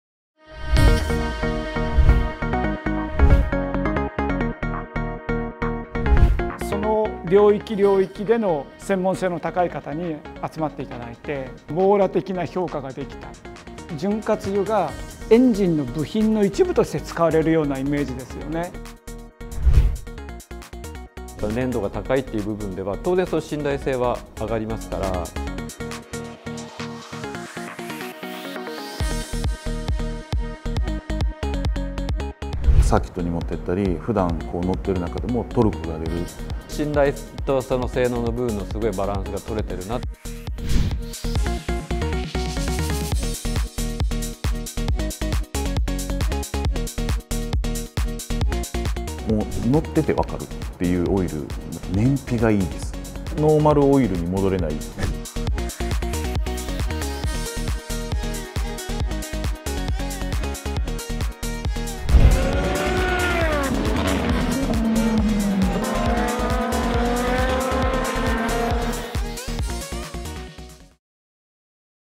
MUGEN×MOTUL ハイパフォーマンスエンジンオイル MS-P 0W-30
エンジンの《MUGEN》×オイルの《MOTUL》
各領域のプロフェッショナルが評価を重ね、幅広い車種に対応するハイパフォーマンスオイルを開発！

SUPER FORMULAで培った技術を織り込み開発したこのオイル。
ピストンリングの気密性を向上させ、エンジンの低回転域からピークパワーを発揮、ドライバビリティの向上に貢献。

燃焼ガスの吹き抜けも低減し粘度低下を防ぐ事でターボチャージャーやエンジンを保護します。

Nシリーズ・e:HEV車・SUV・ミニバン・スポーツ
全ホンダ車にオススメ！

高性能オイルによる「滑らかな運転」＆「エンジン寿命の向上」を
是非、あなたの愛車でご体感ください！

開発に携わった株式会社M-TEC 田中 洋克と N-ONE OWNER'S CUPにも参戦中のHCM Sports Garage 岩間 浩一氏、設計者であるMOTUL R&D株式会社 新井 克矢氏による開発秘話やインプレッションの対談動画はこちら↓↓

■MOTUL Japan
・HP

■HCM Sports Garage
・HP

■無限オフィシャル
・HP